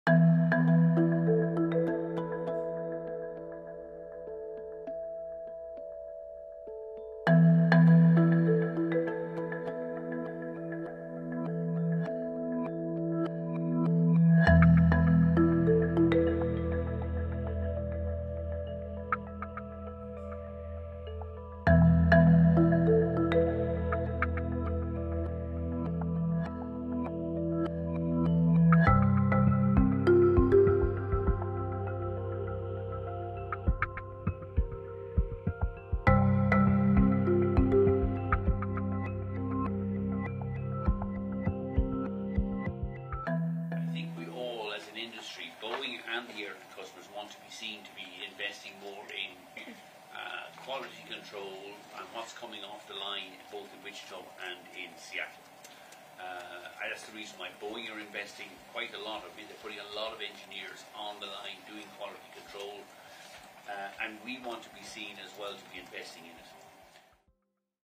Ryanair to step up oversight of Boeing after Alaska Airlines mid-air blowout accident
Ryanair will deploy more engineers to oversee quality control at US factories building Boeing aircraft for the airline.

Michael O’Leary, group chief executive at Ryanair, said the carrier has agreed to step up its activities at the manufacturer’s site in Seattle and Spirit AeroSystems in Wichita, where fuselages are built.

This comes after a Boeing 737 Max 9 operated by US-based Alaska Airlines suffered a mid-air blowout on January 5.